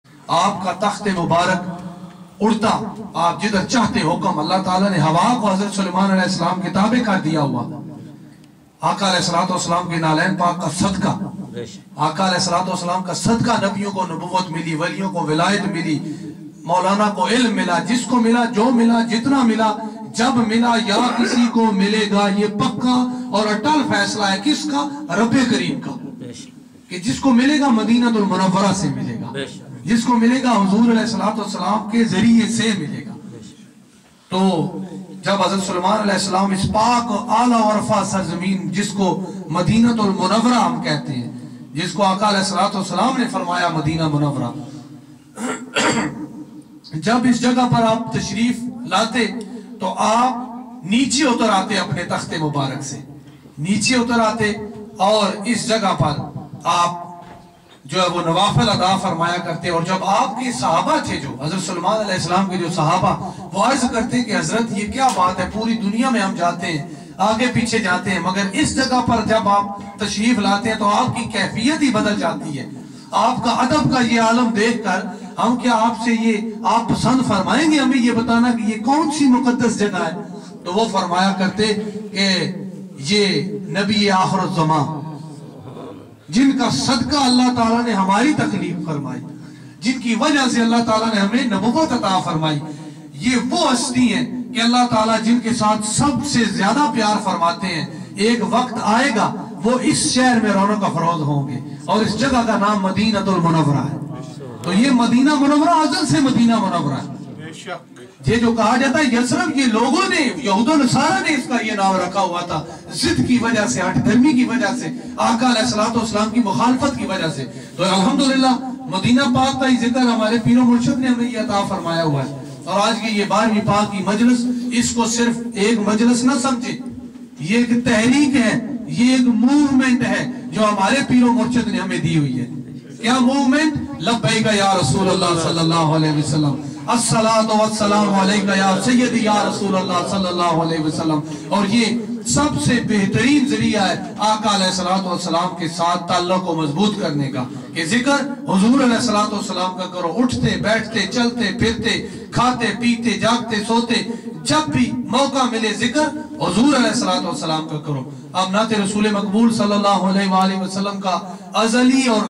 Monthly Barweeh Paak Speech by Khateeb ur Rehman Sb P1
Hosted by Janab Haji Mahboob Sahib Elmwood Rd Slough Uk Khudaam-e-Eidgah Sharif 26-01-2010 Kharota Madni